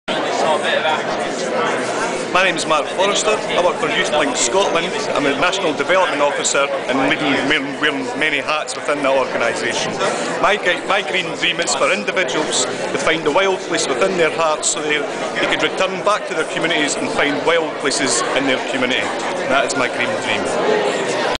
Marc wants us to find our wild places
Marc Forrester dreams of individuals uncovering their wild place within themselves, so they can discover the natural wild place in their community, and in turn care for nature.  Marc is the National Development Officer for YouthLink Scotland.